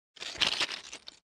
Footstep on the train stones slow 15 - HQ sound effect
Footsteps on the train stones, slow steps, single step.